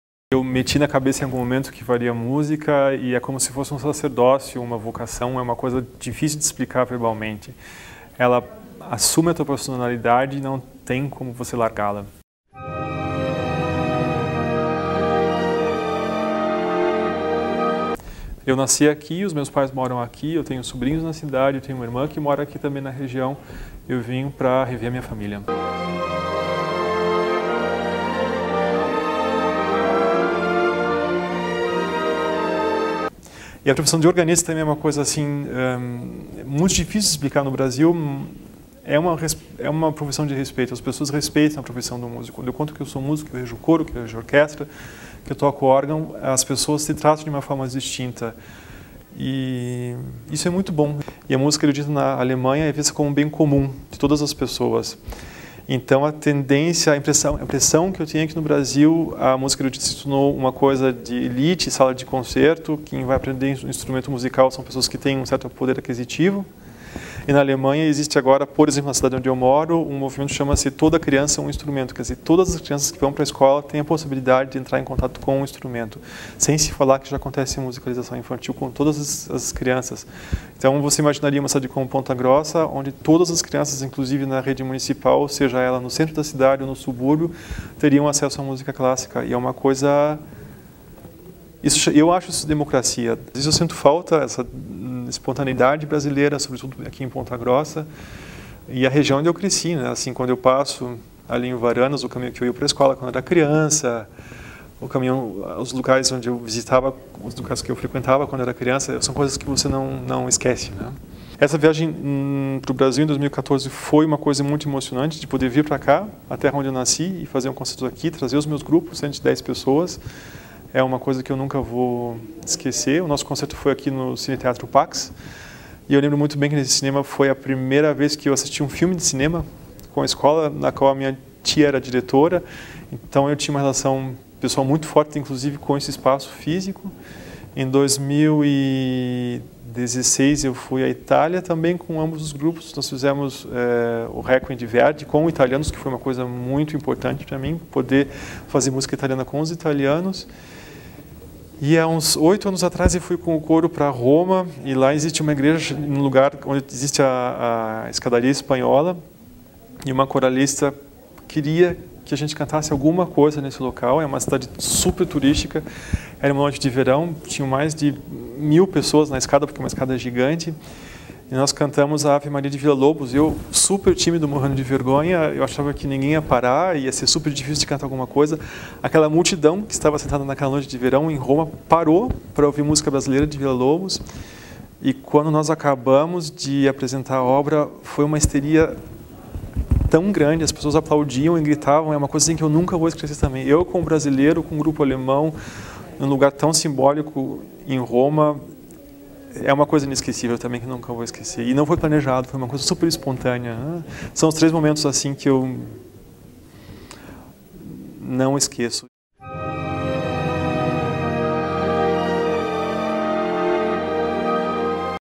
Entrevista com o músico Fernando Swiech
Conversamos com Fernando Swiech, maestro pontagrossense que vive em Hamburgo há mais de 17 anos. O músico conta um pouco de suas experiências e memórias, na Alemanha e em Ponta Grossa.